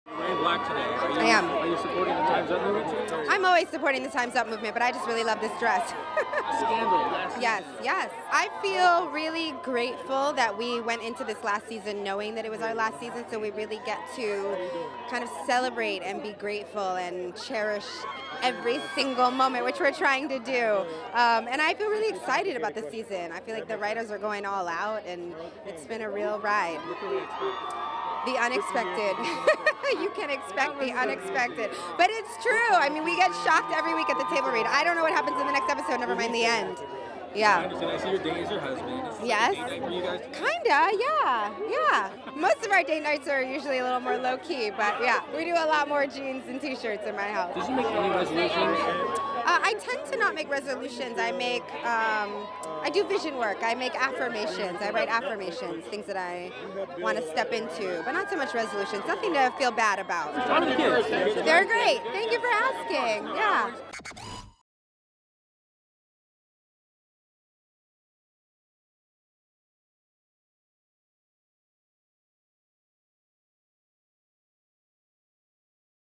Kerry Washington Always Supports Time's Up Movement | E! Red Carpet & Award Shows
From the last season of "Scandal" to date night with her hubby and New Year's resolution, see the "Scandal" star get candid at the NAACP Awards.